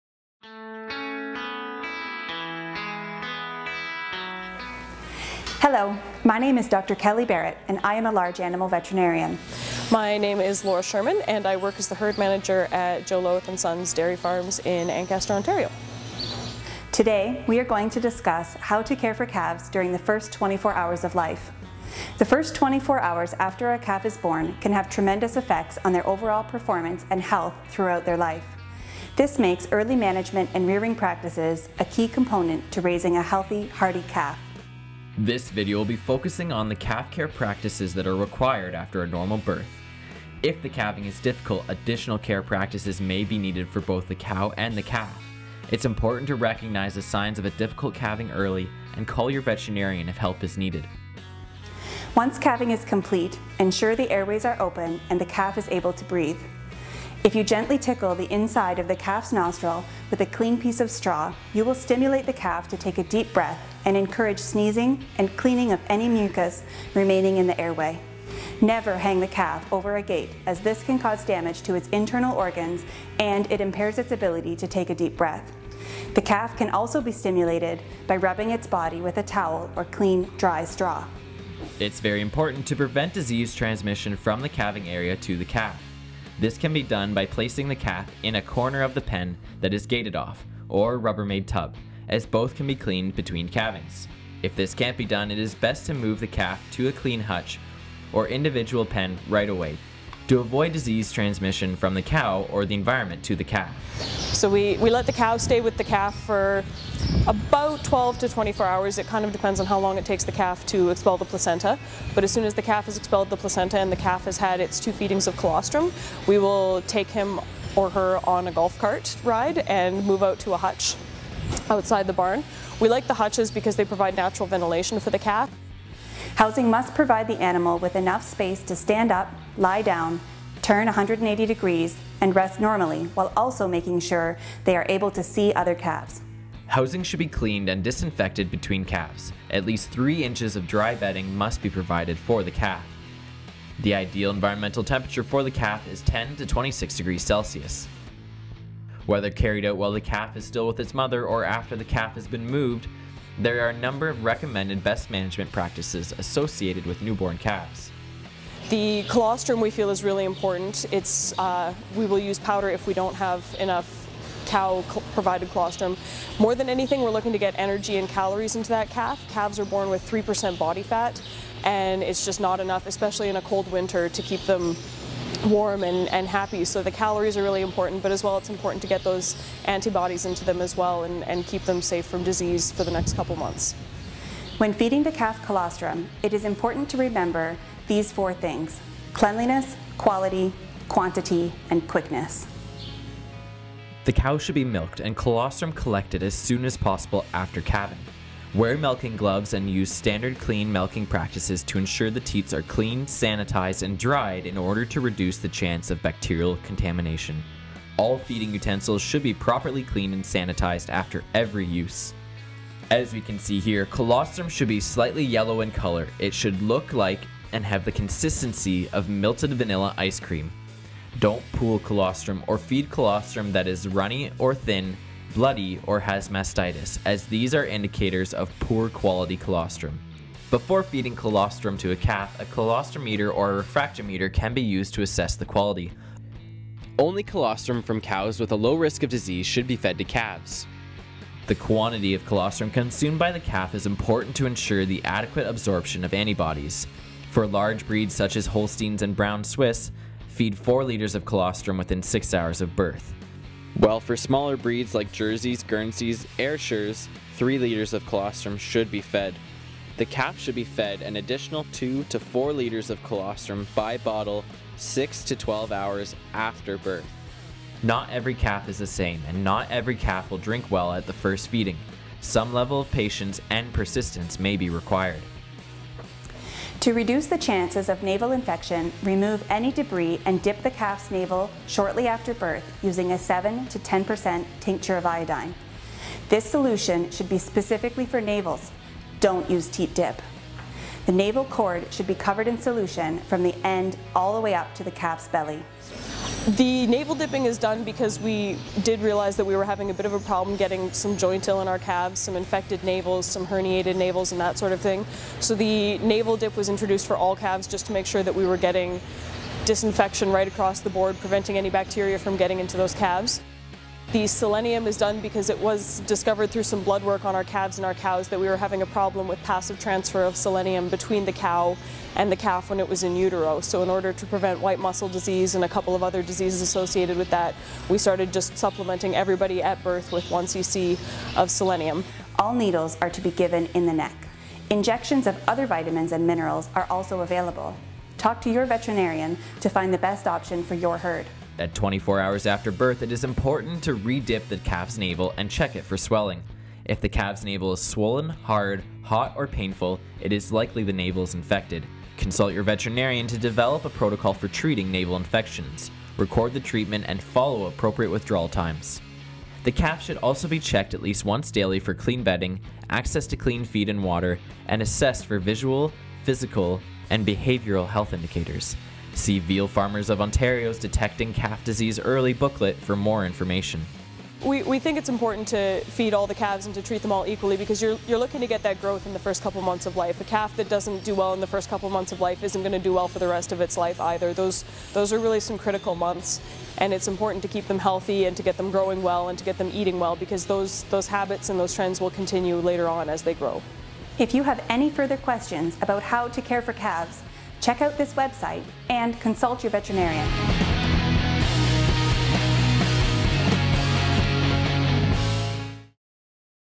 Calf Welfare in the First 24 Hours
As part of the "On-farm animal welfare producer education series" project, Veal Farmers of Ontario (VFO) has created a video showing how to care for calves in the first 24 hours of life.  The first 24 hours after a calf is born can have tremendous effects on their overall performance and health throughout their life. This makes early management and rearing practices a key component to raising a healthy, hardy calf for the dairy, veal and dairy-beef industries.  Animal care is a top priority for VFO, and through new research projects, innovation and education, we continue to work with producers to find solutions to their on-farm issues and improve calf health in the province.